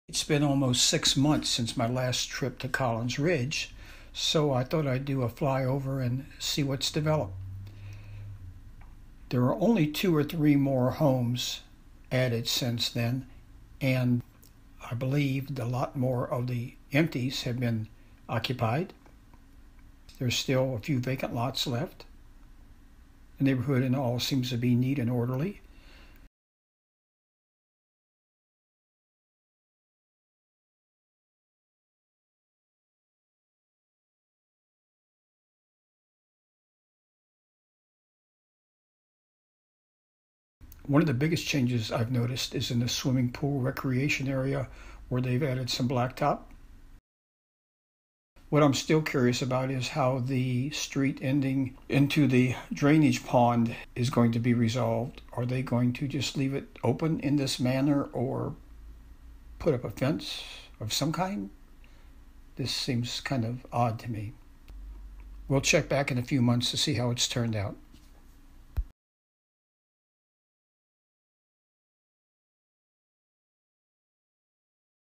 Collins Ridge - Five Months Later, Little New Construction - Hillsborough, NC
Overflight of the Collins Ridge Development with construction currently in progress. In it's final form, it is projected to be a 1,000 unit subdivision in Hillsborough, NC.